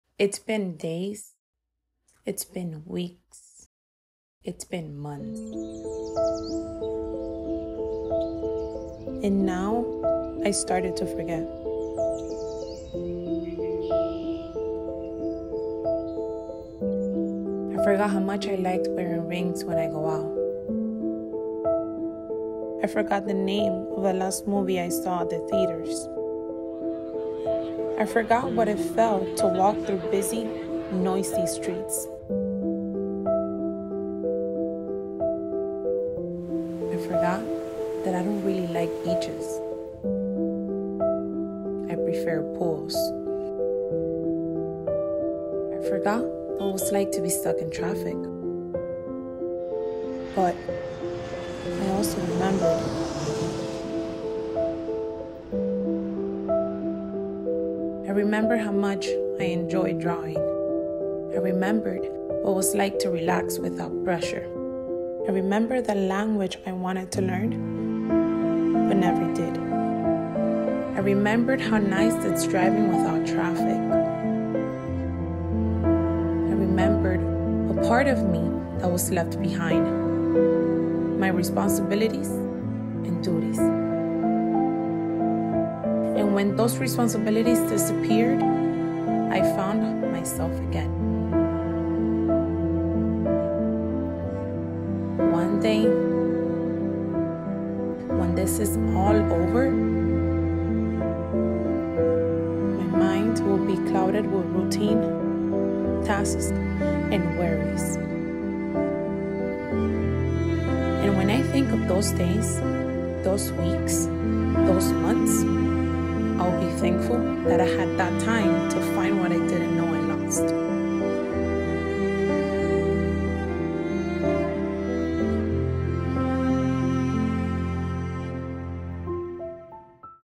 Memory, short film by Marcela Lugo
a poem about the positive side of quarantine due to the coronavirus pandemic.